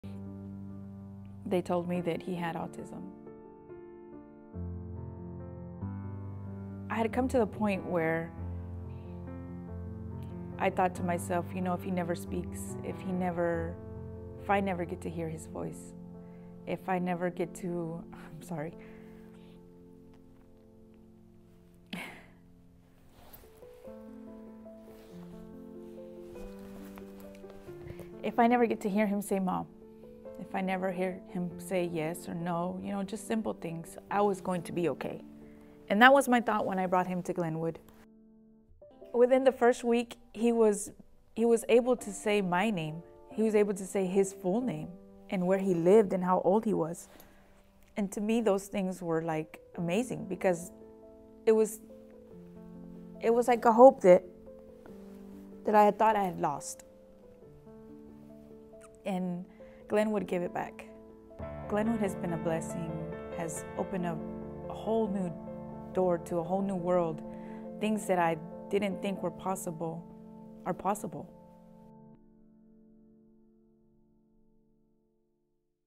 Meet Ian & his family.
Ian is a child served by Glenwood who has graduated from our Children's Center